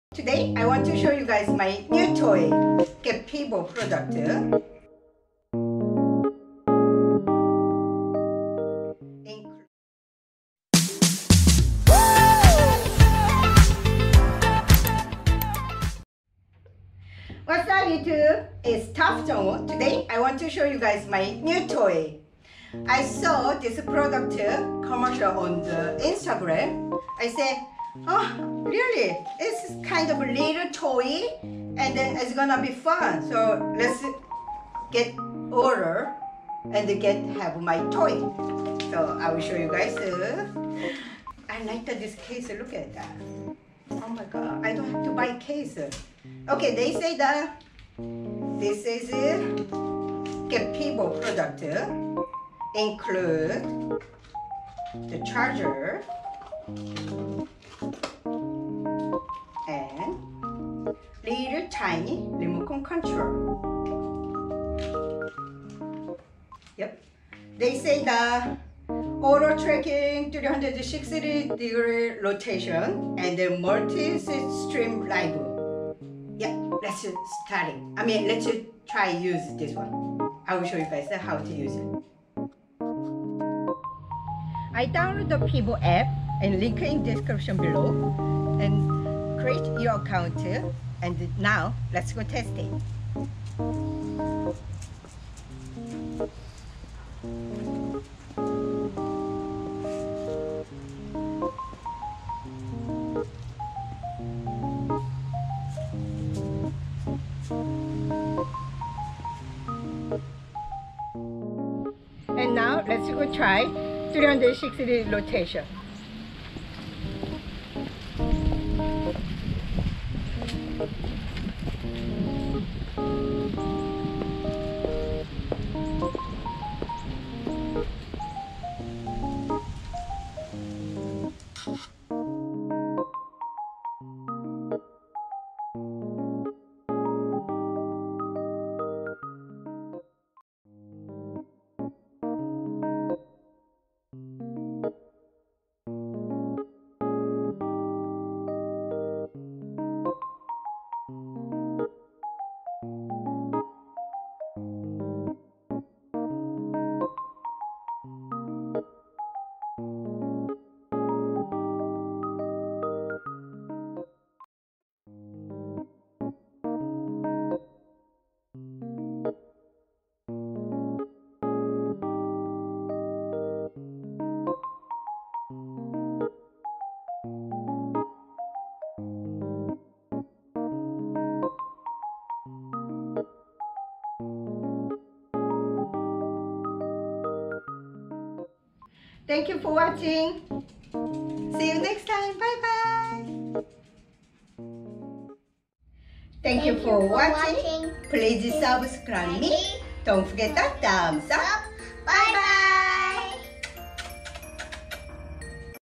[JWYao TV] Simple review of PIVO.
Simple review of PIVO.
Download the pivo app.
create your account and follow the direction.

이번에 새로운 제품을 하나 구입했습니다.
아주 간단한 리뷰를 해봤어요.
아직 리뷰를 어떻게 잘 해야하는지 몰라서
이번엔 아주 어리버리하게 한 것 같네요.ㅎ
담엔 더 잘 하겠습니다! ㅎ